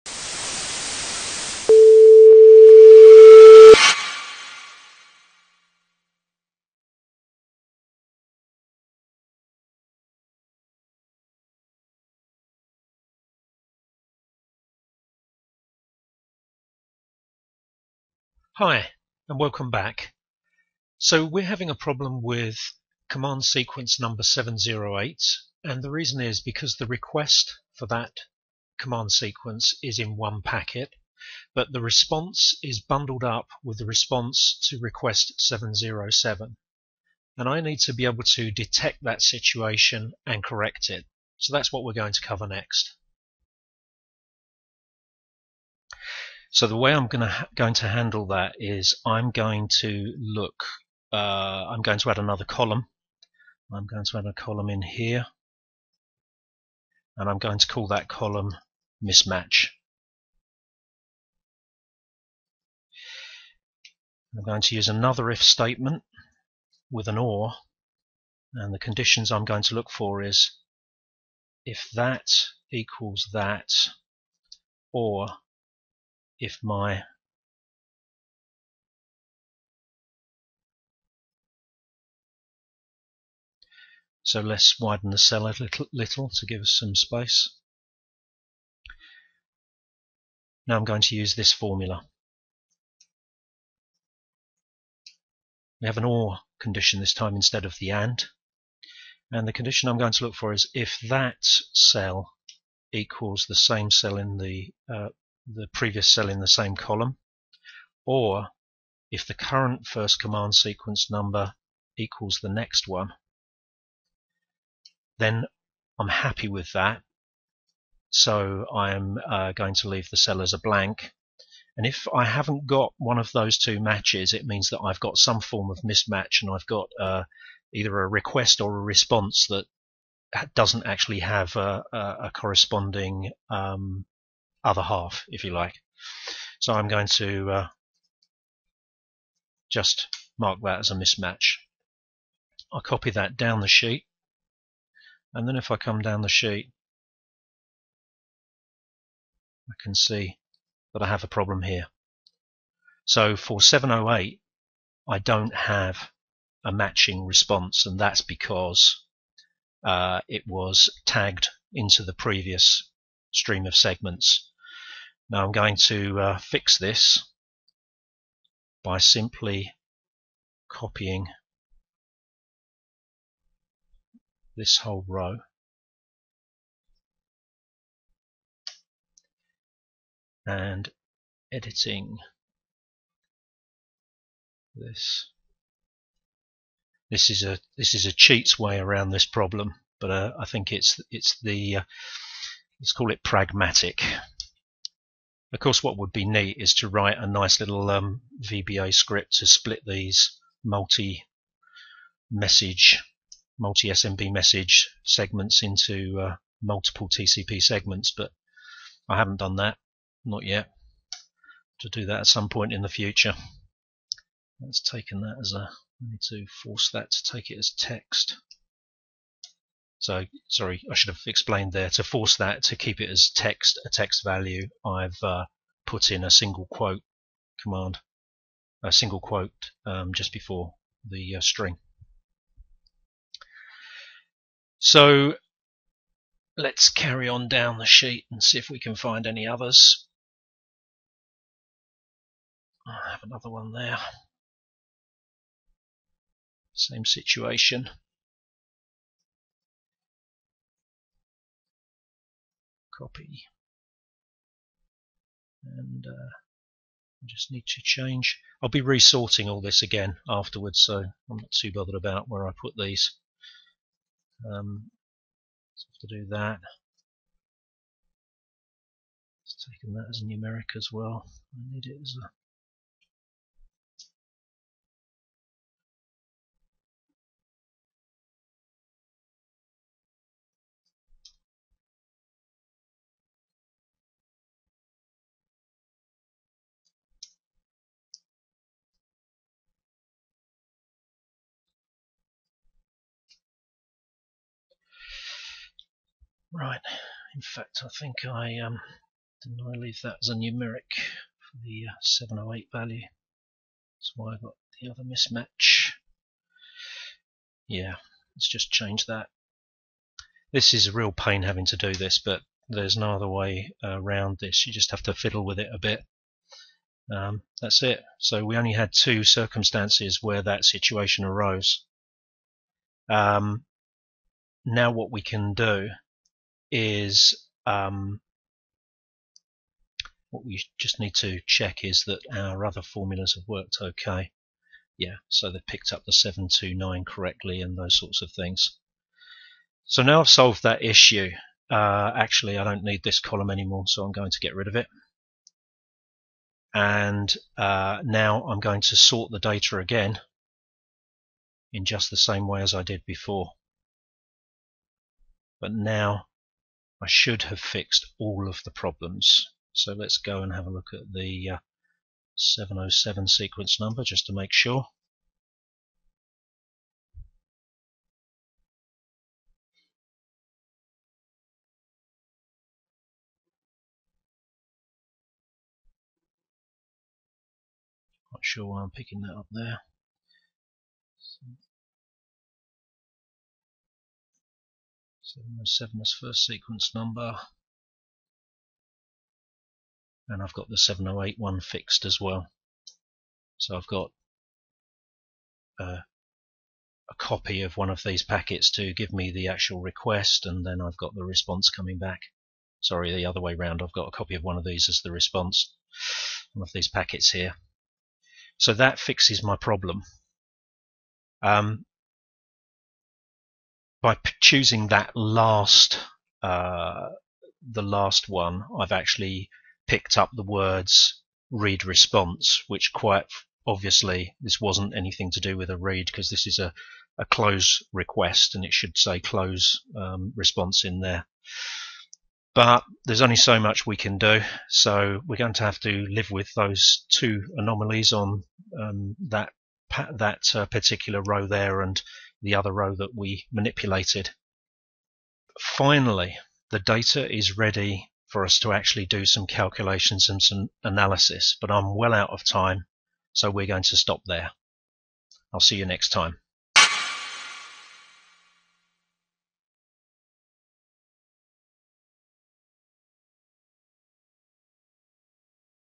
RPR NA02: Analysing SMB2 and fileserver performance - Part 5
In this series of videos we study the performance of a Windows 2008 r2 server by analysing the SMB2 traffic flows in a network trace with Wireshark and Excel.

This is part 5 and it covers the final tidying of the spreadsheet data prior to analysis.